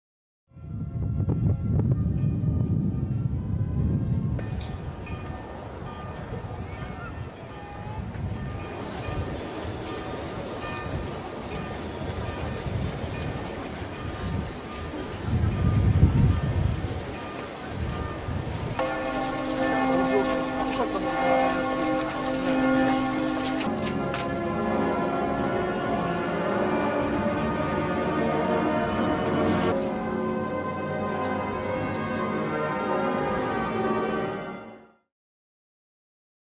Thousands march beside coffin of murdered Gdańsk mayor
Carrying flags and candles through the streets, thousands of people walked in procession on Friday along with the coffin of Gdańsk's murdered mayor, Paweł Adamowicz. He was stabbed on Sunday evening by a man who rushed the stage during a charity event. Adamowicz died the following day.